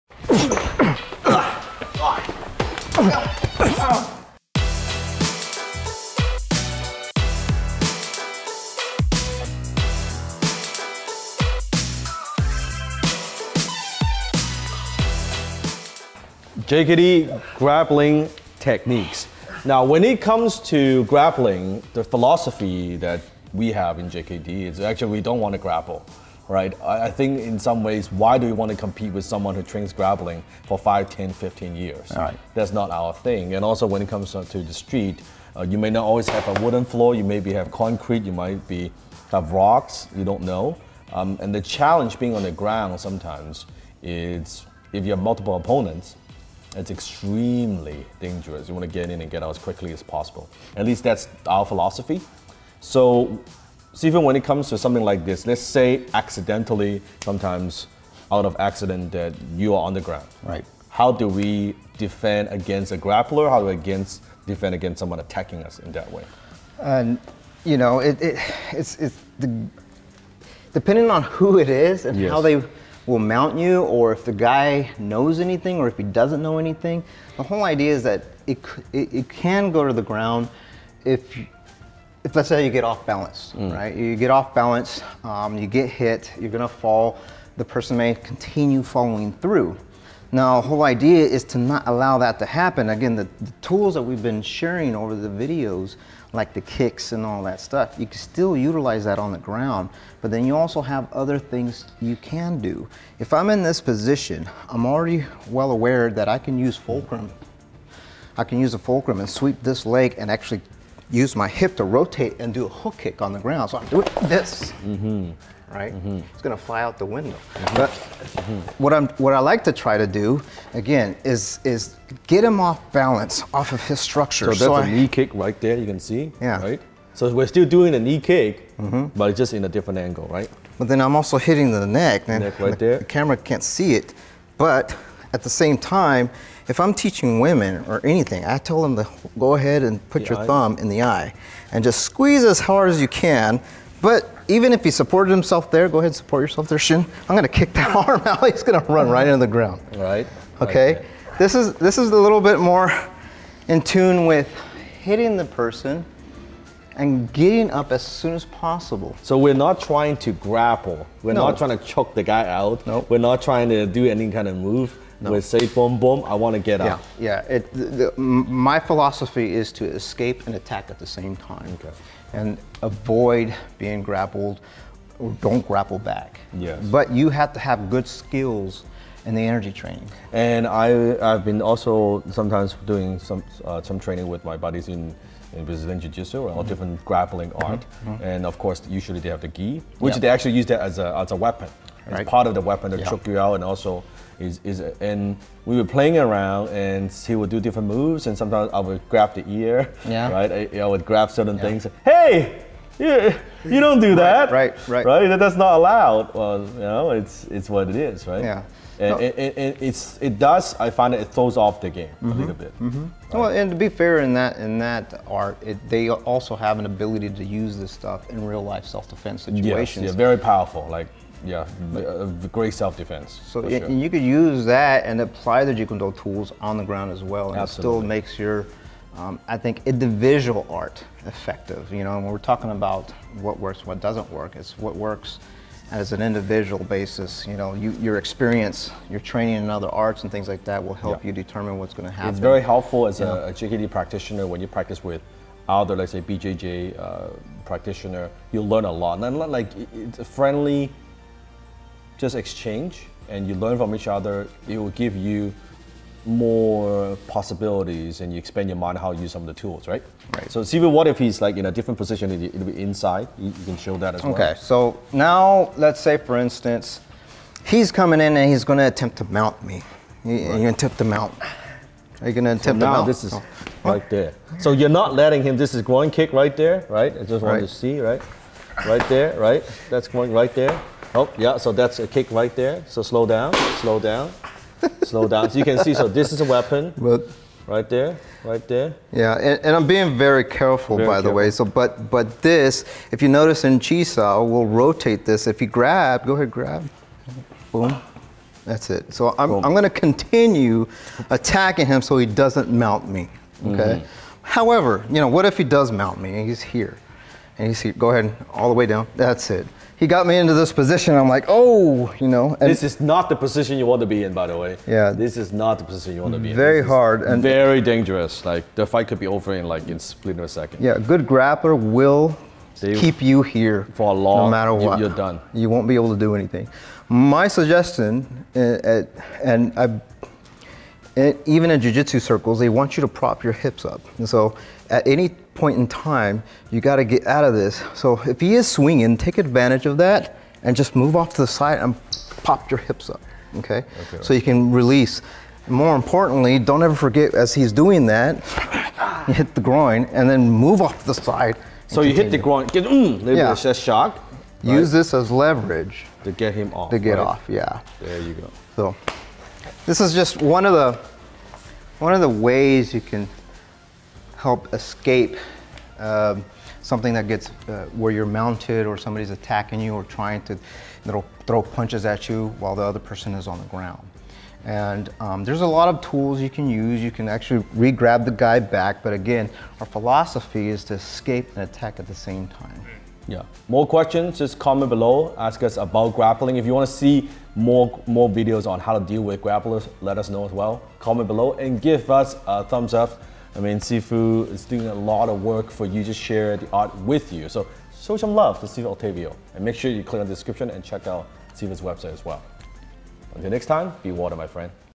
Bruce Lee's Grappling Techniques
How does Bruce Lee deal with grappling? In this video, Sifu Dan Lok and Sigong Octavio Quintero show you how to attack while escaping and Bruce Lee's grappling techniques.